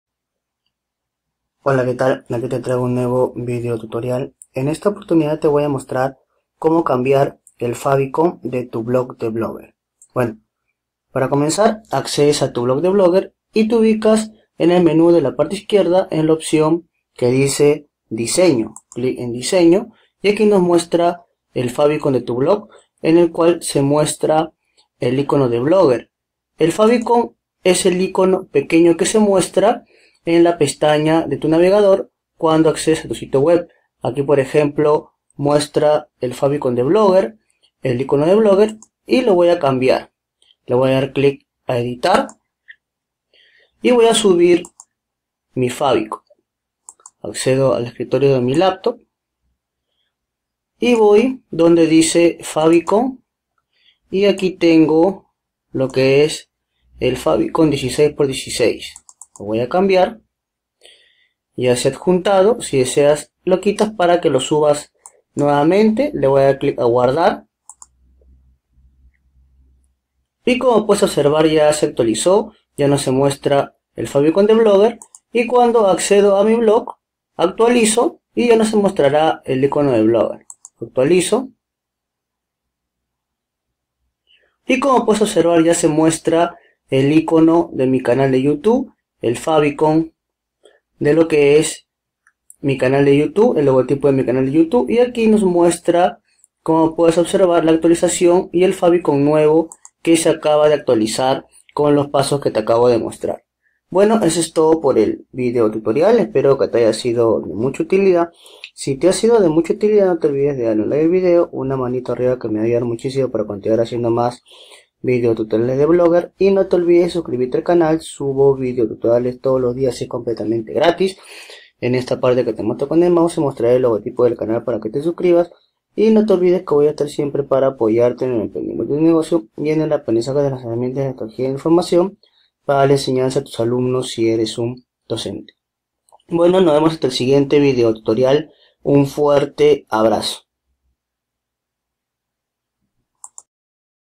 Cómo Cambiar el Favicon de Blogger
Tutorial que detalla como cambiar el favicon de Blogger paso a paso.